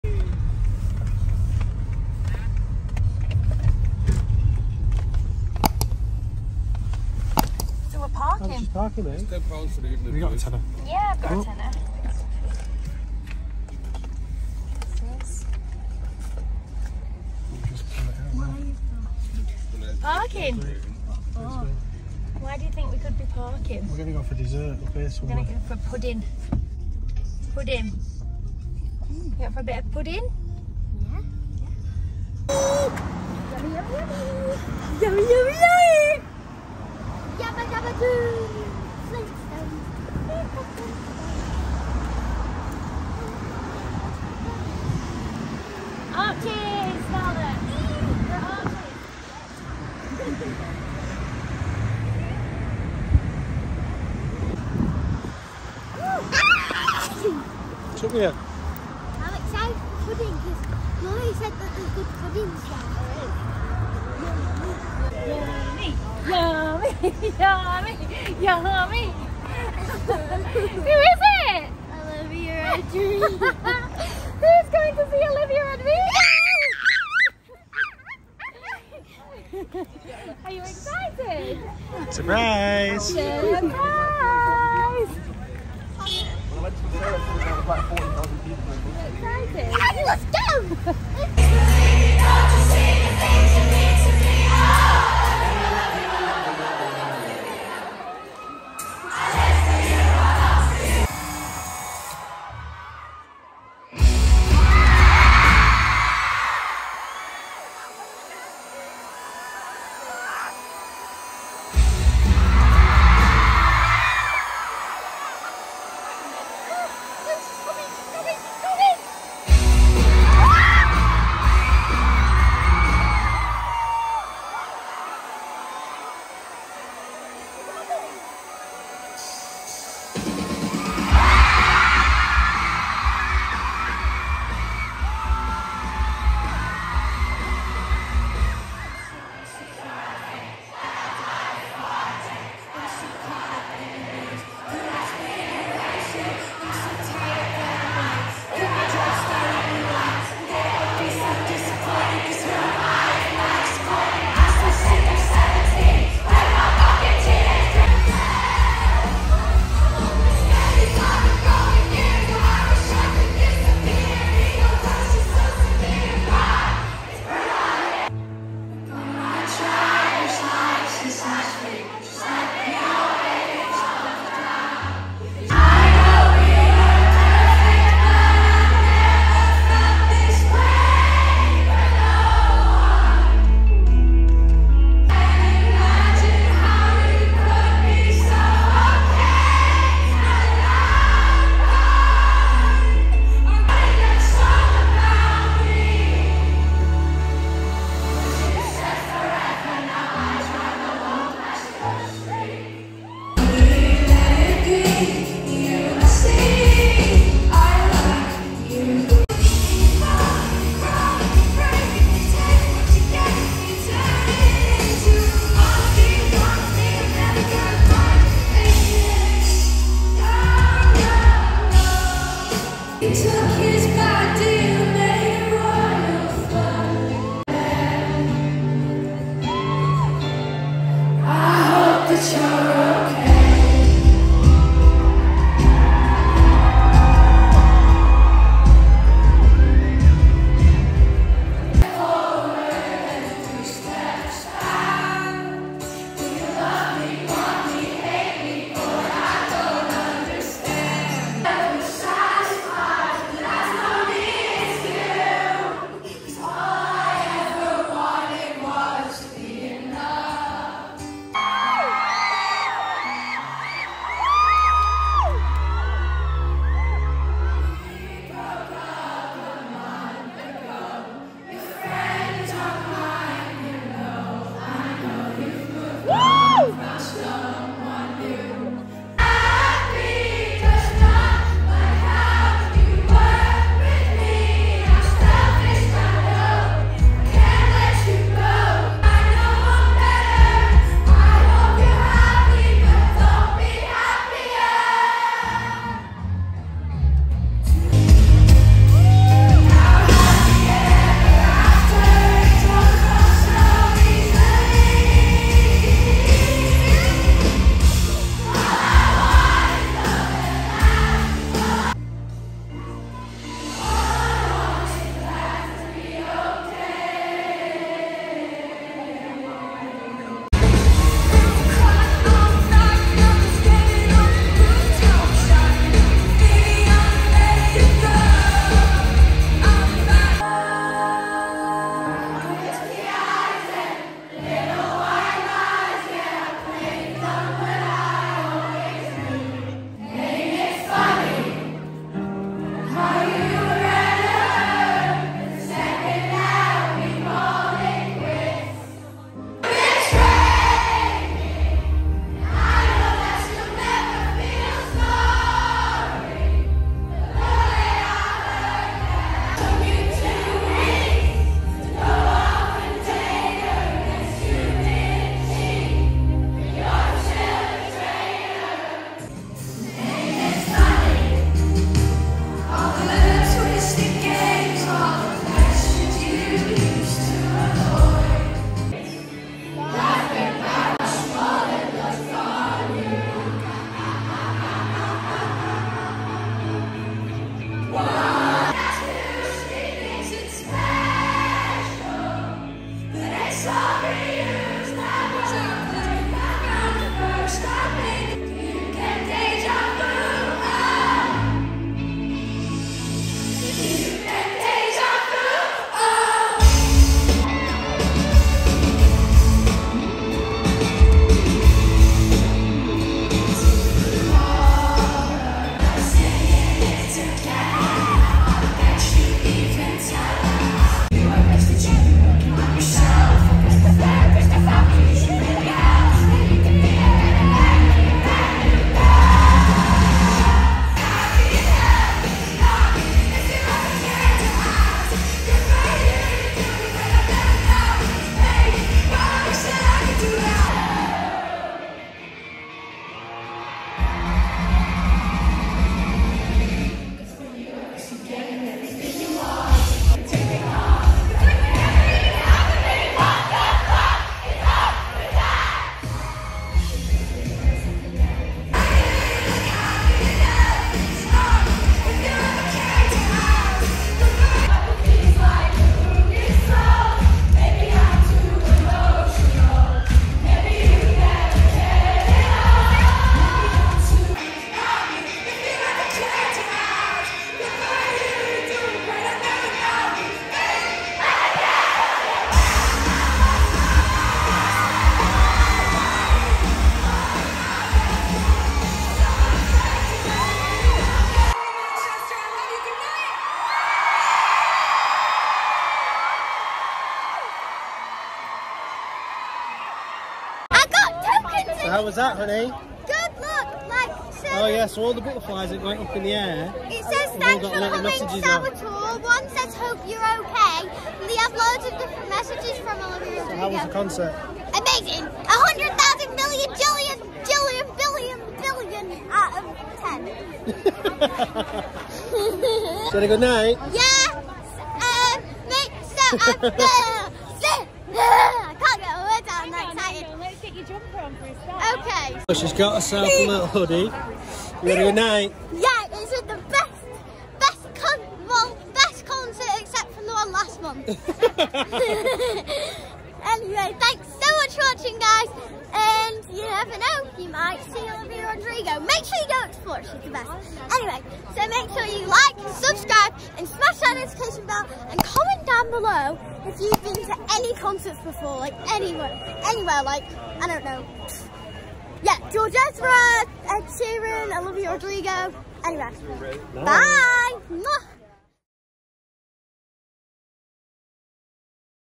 SURPRISE Concert for Scarlett to see OLIVIA RODRIGO at Manchester Apollo
We surprised Scarlett with a trip to watch her favourite artist, Olivia Rodrigo at Manchester Apollo in July 2022.